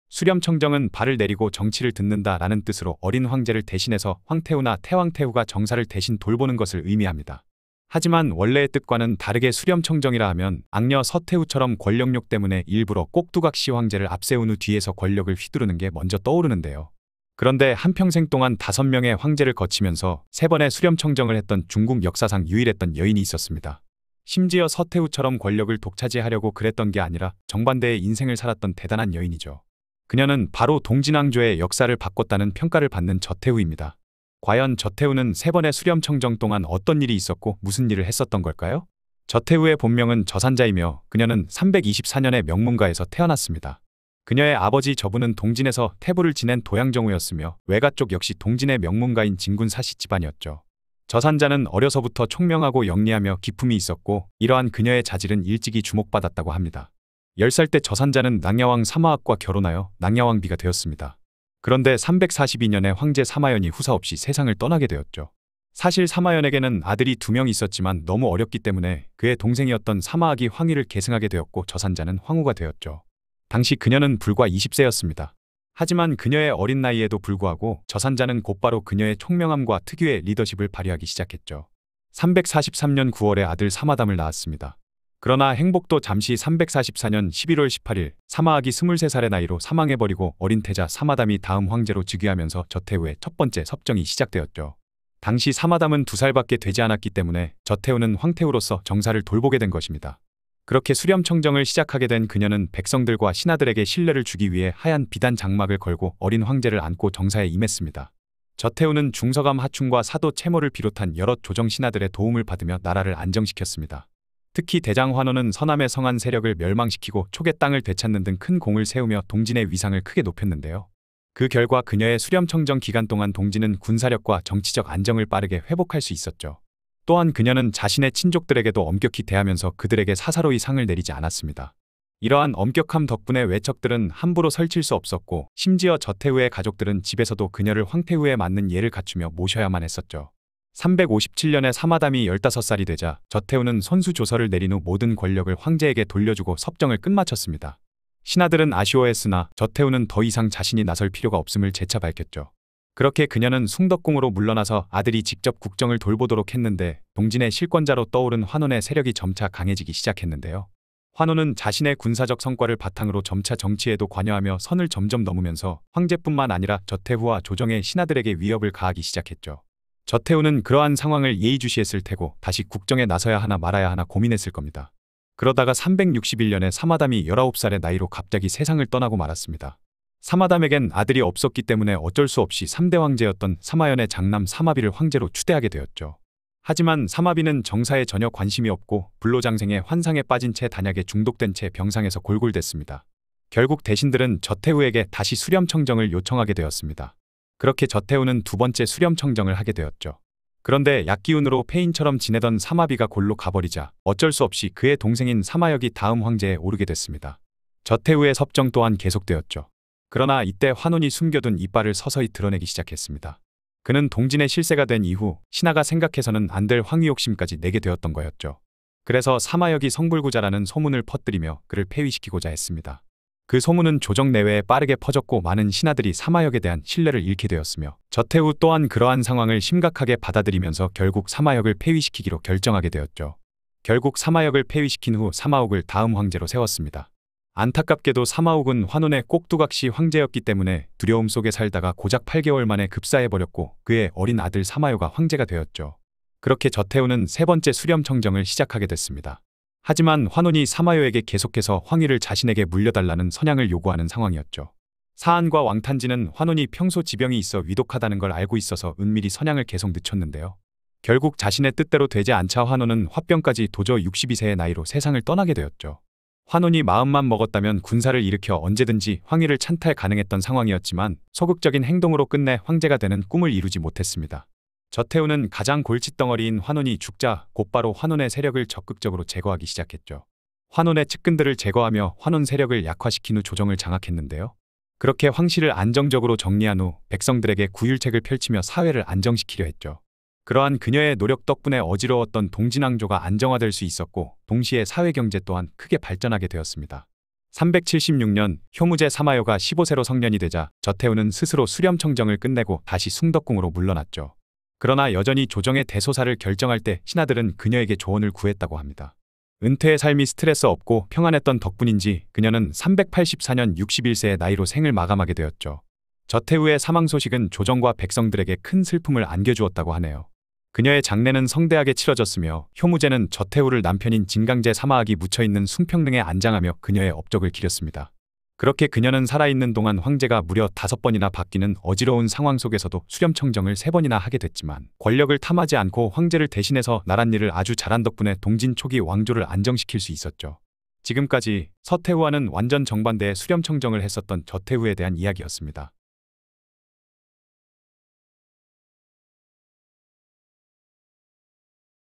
저태후. 무려 3번의 수렴청정을 하면서 동진의 안정과 발전을 가져온 여인
무려 3번의 수렴청정을 하면서 동진의 안정과 발전을 가져온 여인
저태후에 대한 이야기 입니다

감사합니다!